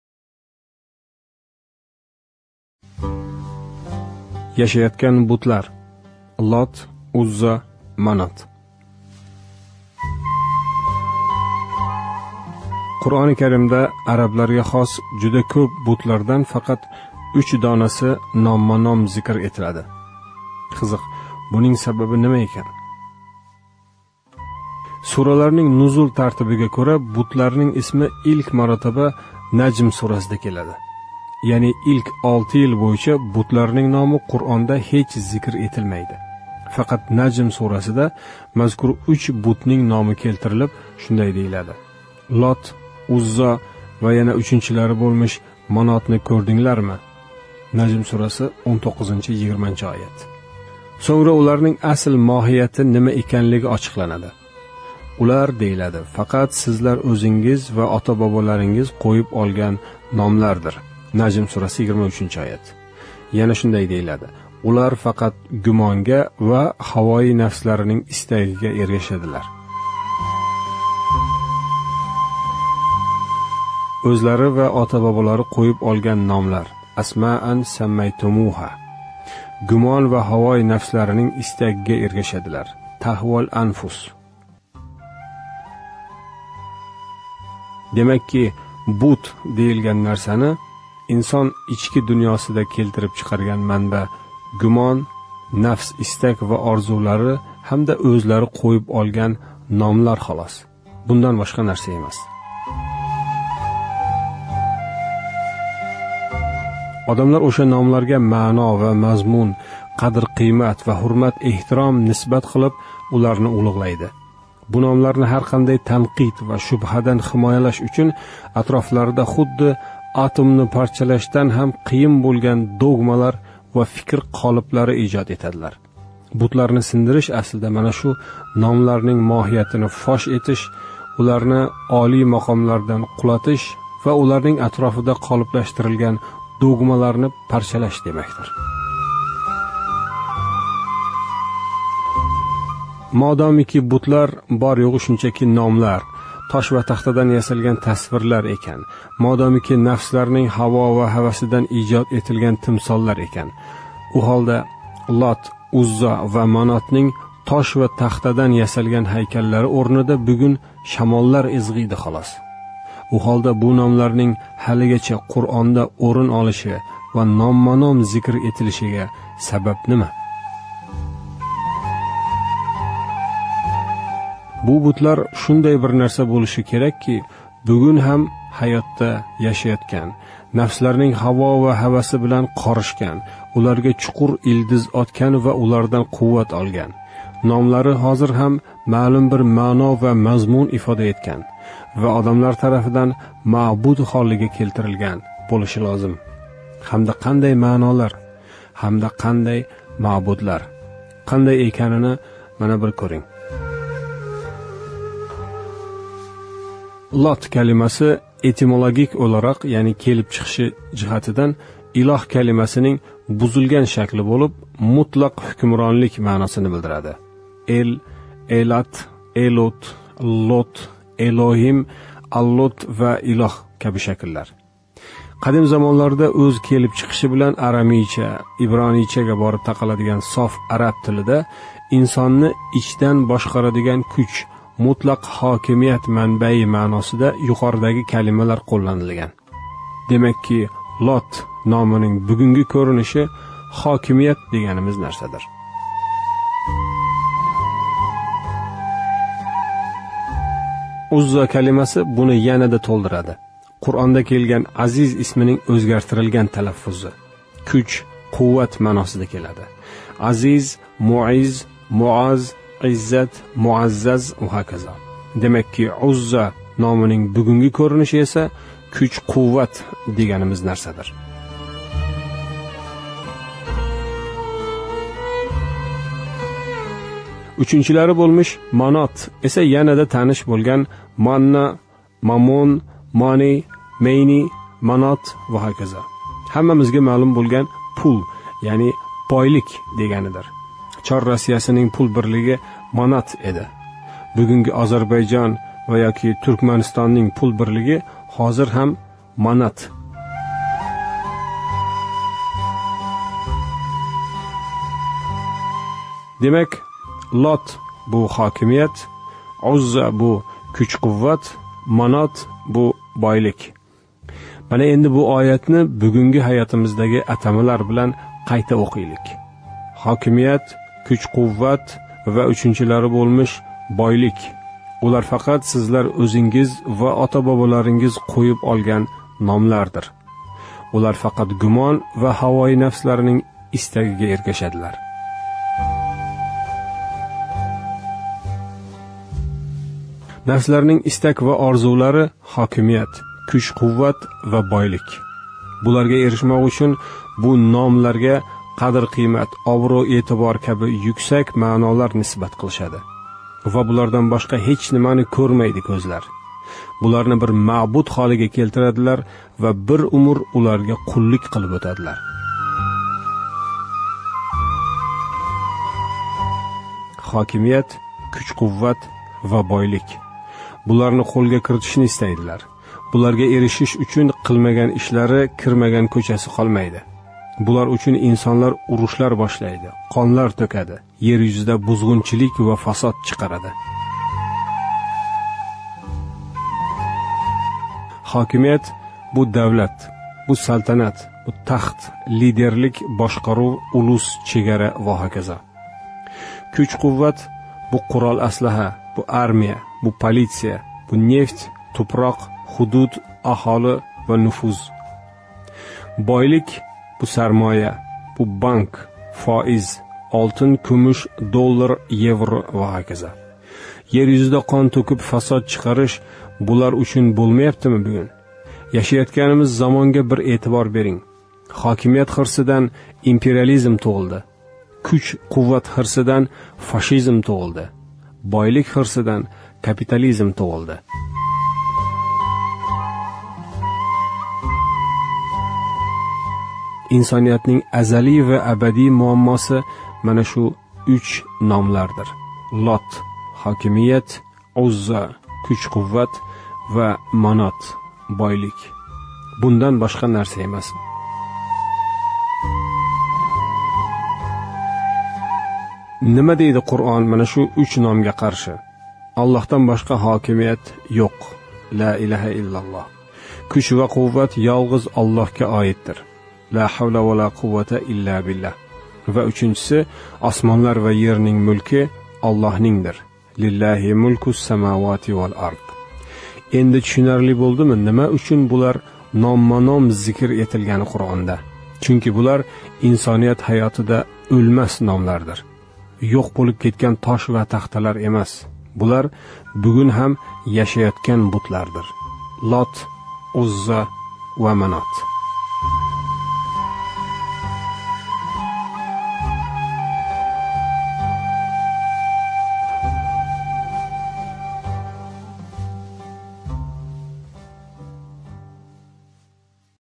ЯШАЁТГАН БУТЛАР: ЛОТ, УЗЗО, МАНОТ
Қуръонда арабларга хос жуда кўп бутлардан фақат уч донаси номма-ном зикр этилади. Ажабо, бунинг сабаби нима?